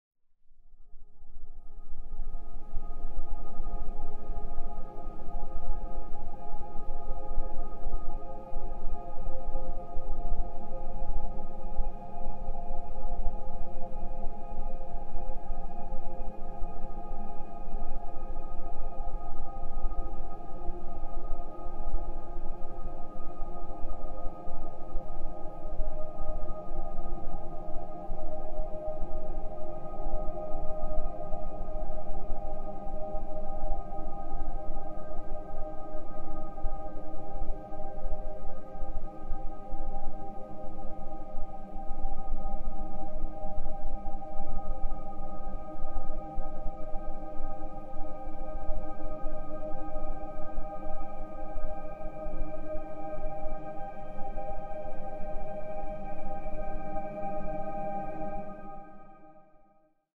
60 Seconds of Zen
Ames Ice Hose. Logan Tyler and Jason Doyle climbing this classic route near Telluride.